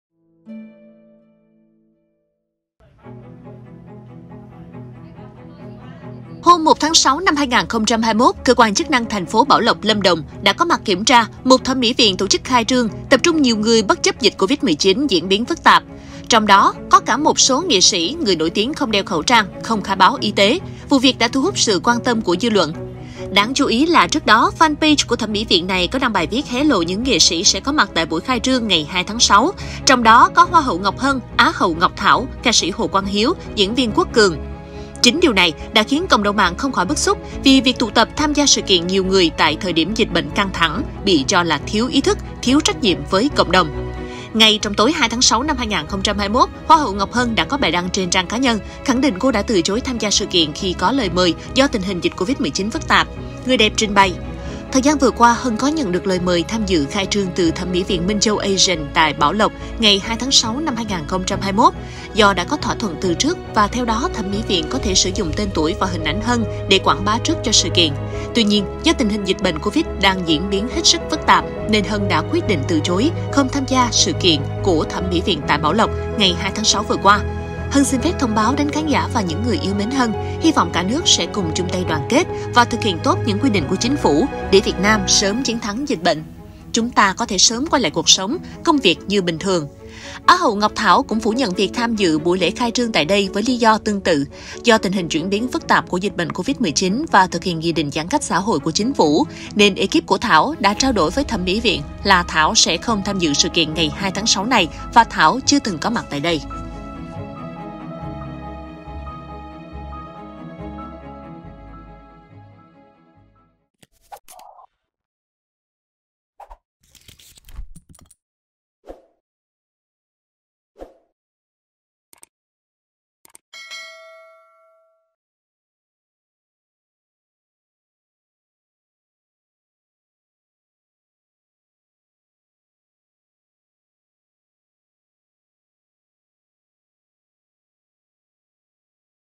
Hai người đẹp làm rõ nghi vấn dự khai trương thẩm mỹ viện bất chấp dịch Covid 19
Hoa hậu Ngọc Hân, Á hậu Ngọc Thảo đã lên tiếng trước nghi vấn dự khai trương một thẩm mỹ viện ở TP.Bảo Lộc (Lâm Đồng) giữa lúc dịch Covid-19 đang phức tạp.